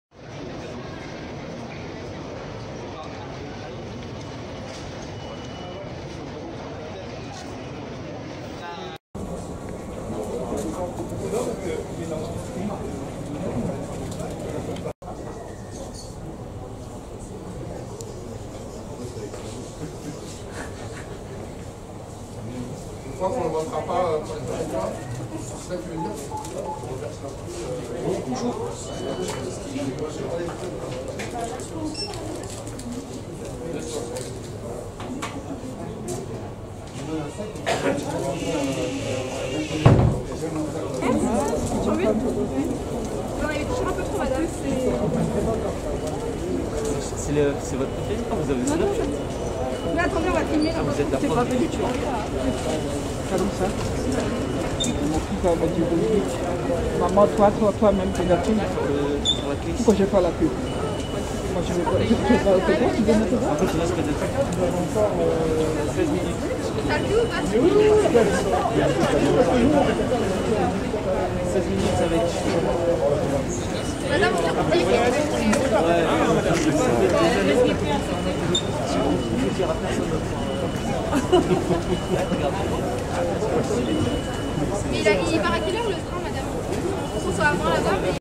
JOEYSTARR en dédicace de son magazine de cuisine au RELAY - 9 décembre 2021 / Suprême NTM Joey Starr
JoeyStarr en dédicace de son magazine de cuisine / magazine culinaire Five Starr au RELAY gare Montparnasse le 9 décembre 2021 / ex Suprême NTM - Joey Starr